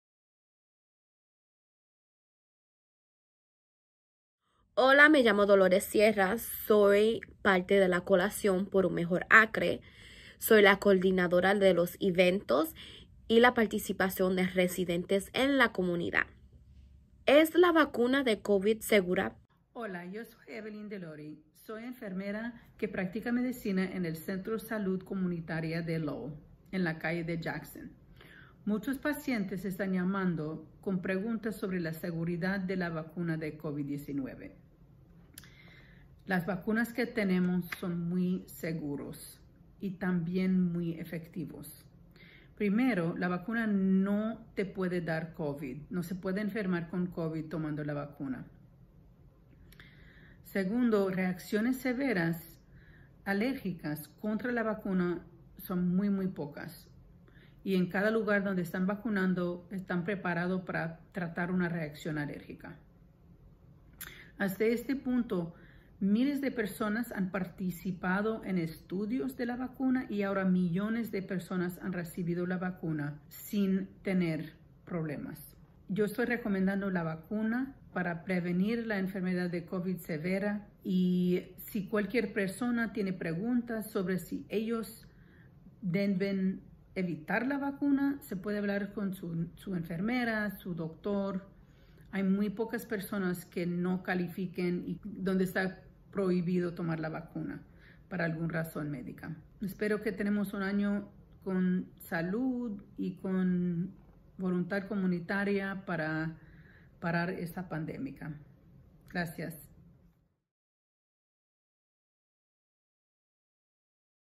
¿Son seguras las vacunas COVID-19? ¡Sí! (Are COVID vaccines safe? Yes!)
¿Son seguras las vacunas COVID-19? ¡Sí! La enfermera practicante del Lowell Community Health Center, Evelyne Delori, explica por qué las vacunas COVID-19 son seguras y recomendadas. Pregunta proporcionada por Dolores Sierra, de Coalition for a Better Acre.

(Are the COVID-19 vaccines safe? Yes! Lowell Community Health Center Nurse Practitioner Evelyne Delori explains why the COVID-19 vaccines are safe and recommended. Question provided by Dolores Sierra, of Coalition for a Better Acre.)